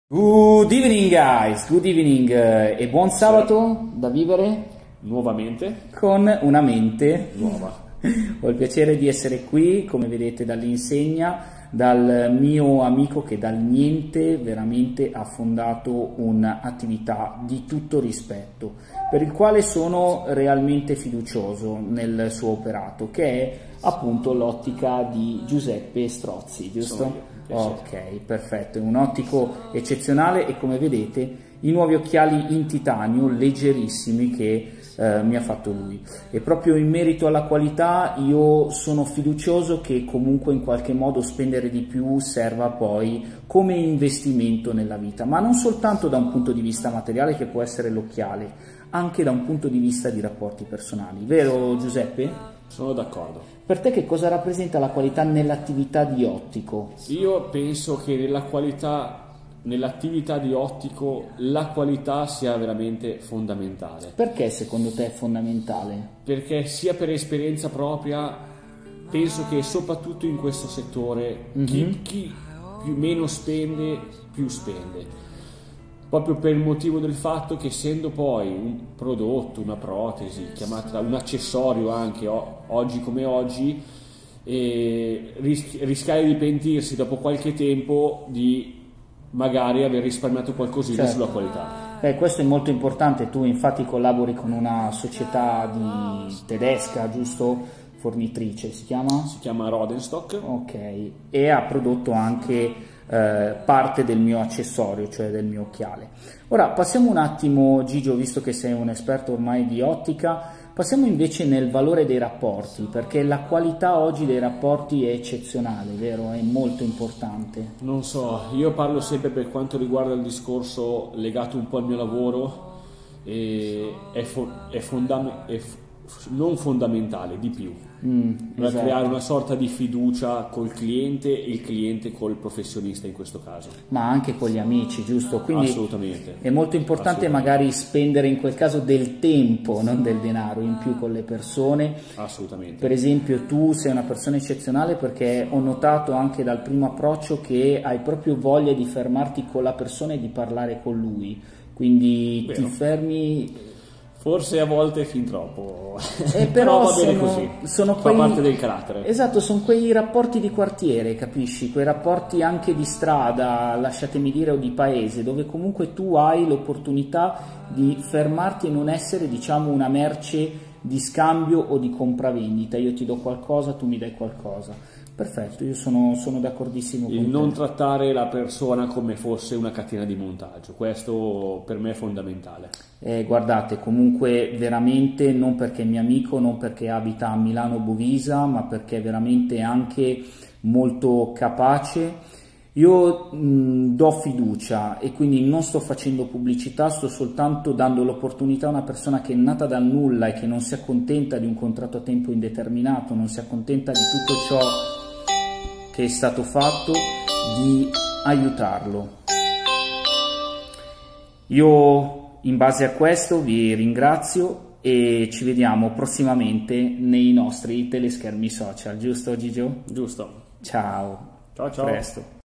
Lezione di Yoga e Consapevolezza nella Presenza: la Qualità dei
Ho il piacere di scambiare sani punti di vista con il mio amico nonché ottico Giuseppe Strozzi in Zona Milano.
A lui un pensiero: "Sono una parte di tutto ciò che ho trovato sulla mia strada"